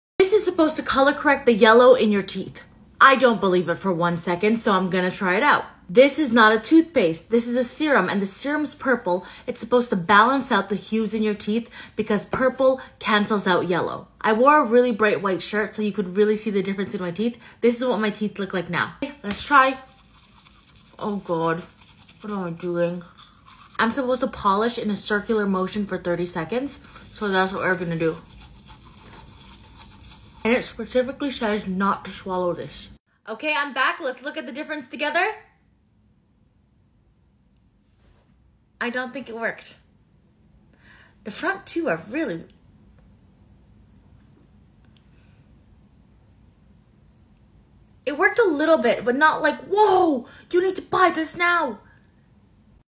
THIS IS SUPPOSED TO CORRECT THE YELLOW IN YOUR TEETH...
Ummmm... I don't think this works...

I get the colour theory idea but I just don't believe it. I'll just stick with Colgate!

T I K T O K :  GLAMZILLA
I N S T A G R A M : GLAMZILLA
P I N T E R E S T : GLAMZILLA